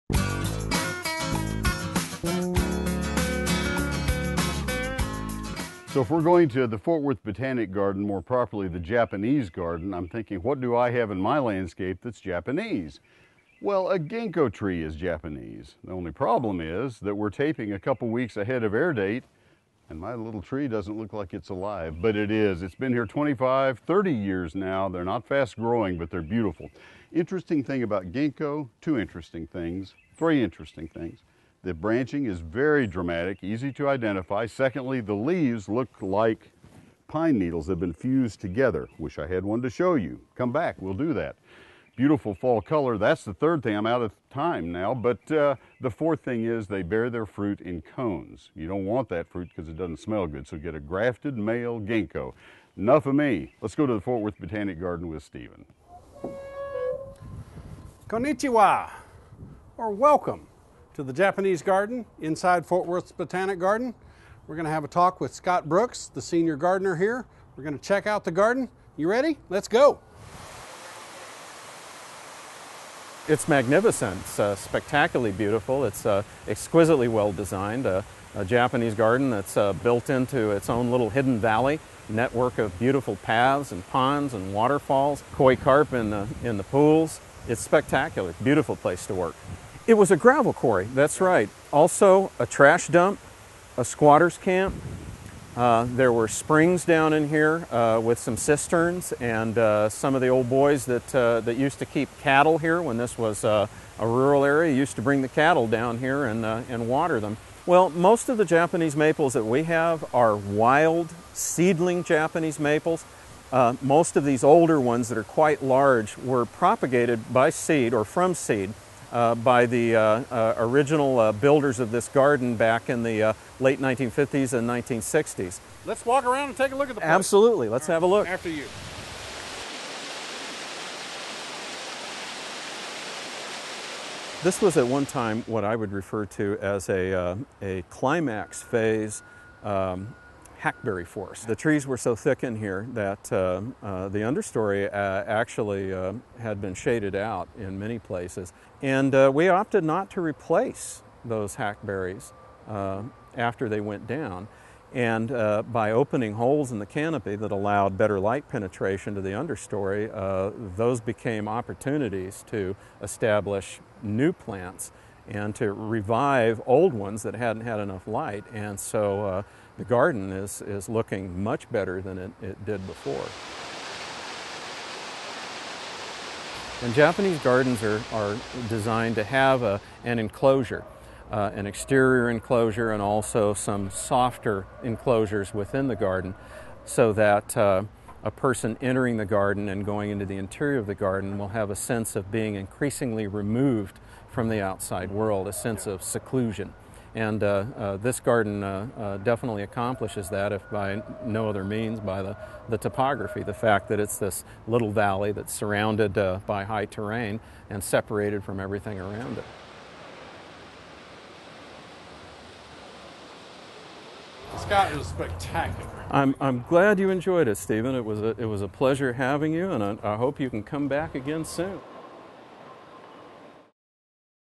Ft. Worth Japanese Garden
Steven takes a tour of the magnificent Japanese Garden at the Fort Worth Botanical Gardens.